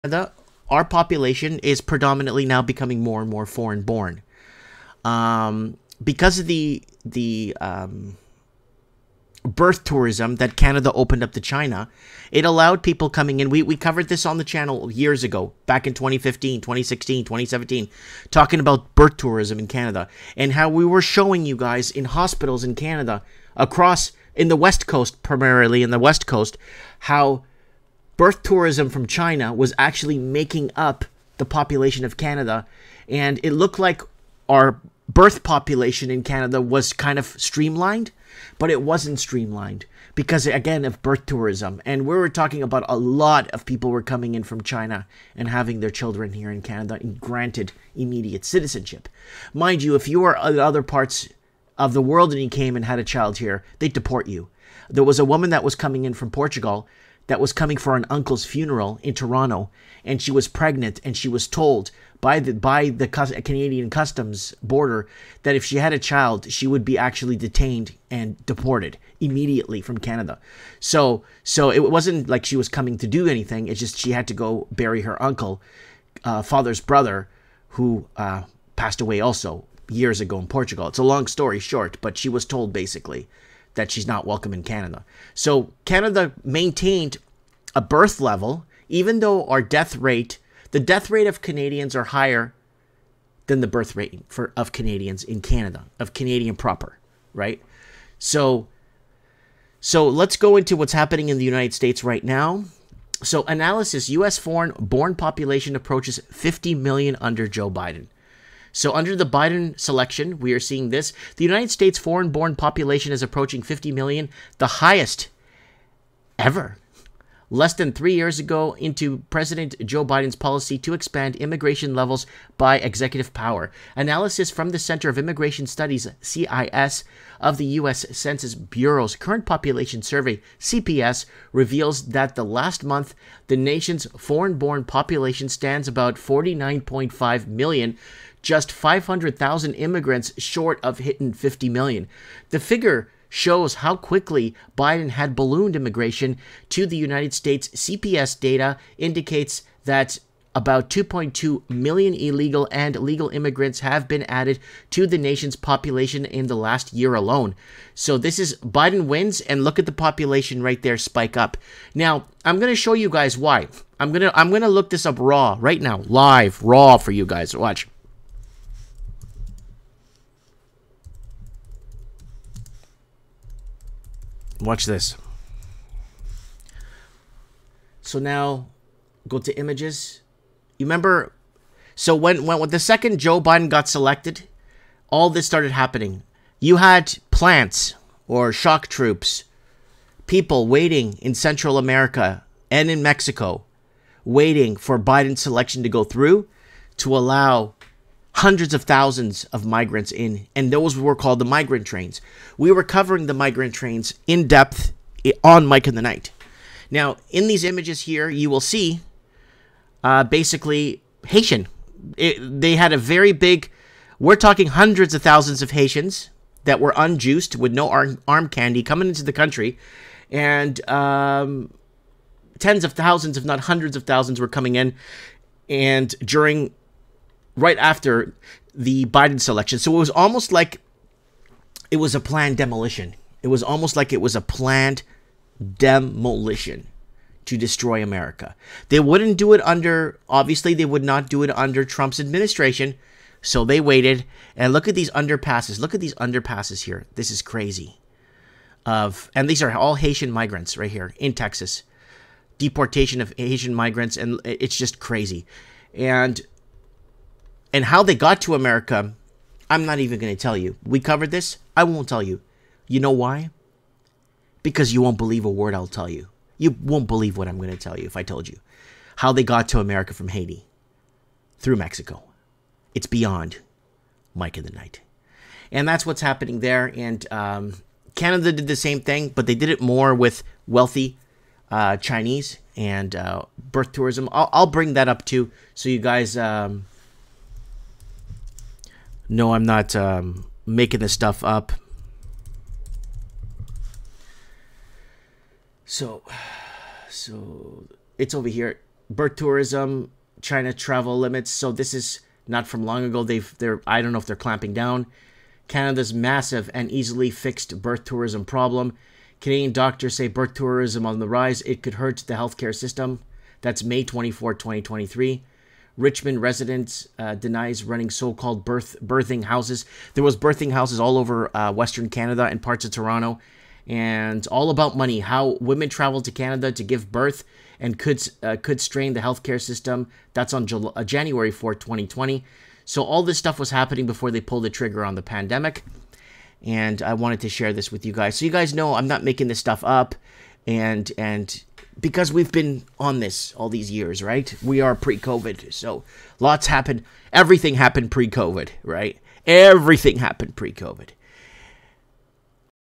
U.S. Foreign-Born Population Approaches 50 Mil, Under Joe Biden, Canada is worse with birth Tourism
The foreign-born population of the United States is on the verge of reaching 50 million, an all-time high, less than three years into President Joe Biden's tenure, during which he sought to increase immigration levels through executive authority.

Analysis conducted by the Center for Immigration Studies (CIS) based on the U.S. Census Bureau's Current Population Survey (CPS) indicates that as of last month, the foreign-born population of the nation stands at approximately 49.5 million, just half a million short of the 50 million mark.

This figure underscores how rapidly immigration to the United States has expanded under Biden's administration. According to CPS data, around 2.2 million both legal and illegal immigrants have been incorporated into the nation's populace in just the past year.